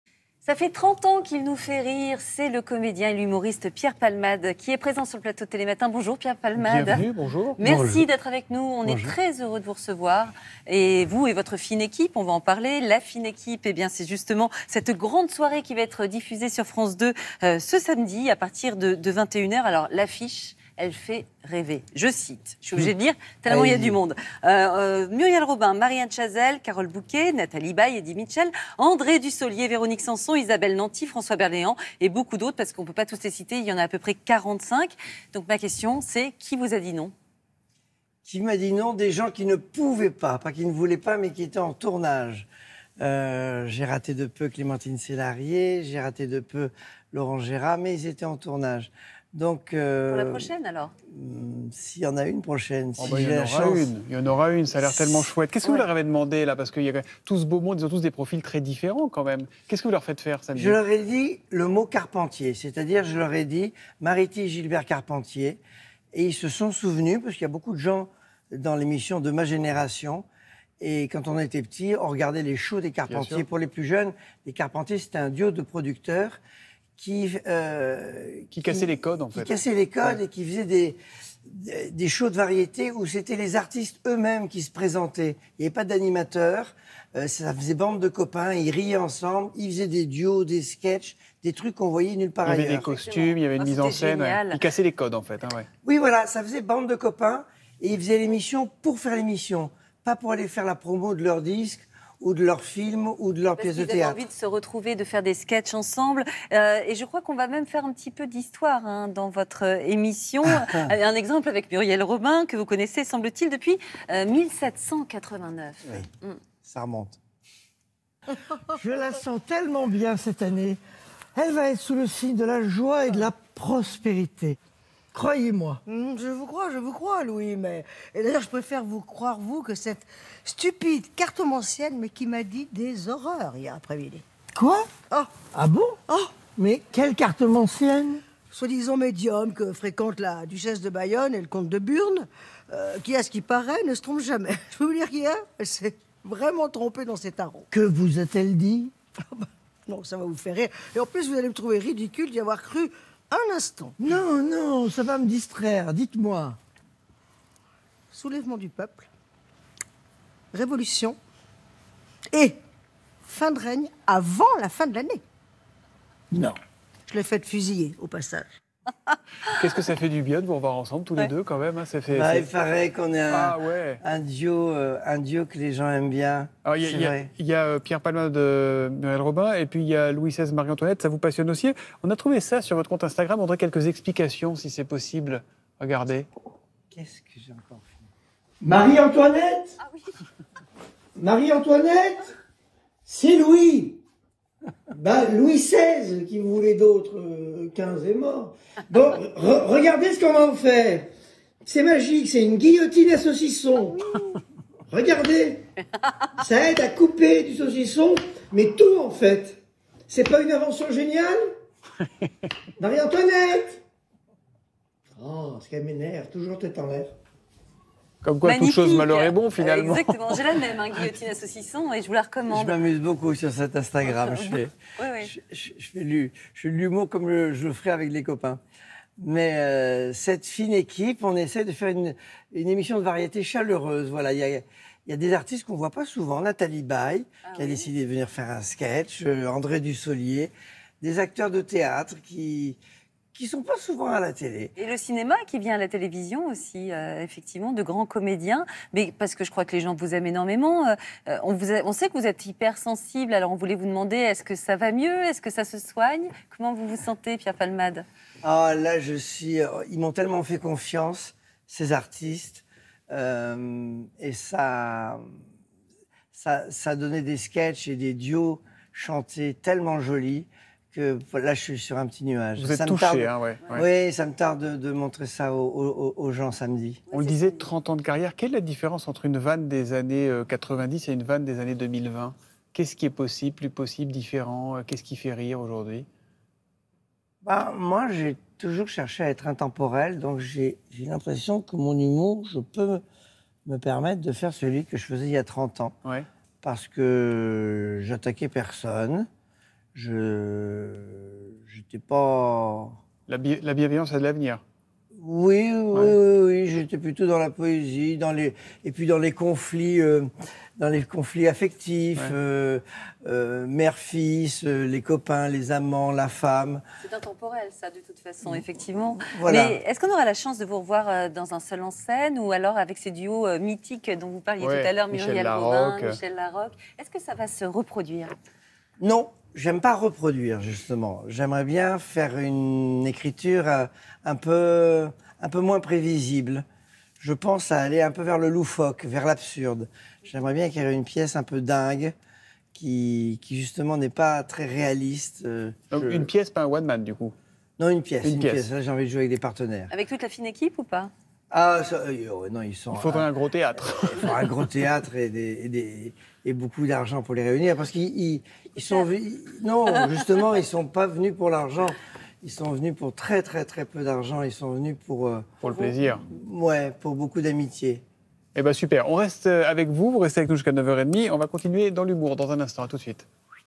L'invité du jour - Pierre Palmade
L’humoriste Pierre Palmade vient nous voir pour nous parler de « La fine équipe », un divertissement orchestré par lui-même et ses amis, dont Muriel Robin, Marie-Anne Chazel, Eddy Mitchell, François Berléand et bien d’autres encore. Une soirée événement à découvrir sur France 2, le samedi 25 juin à 21h10.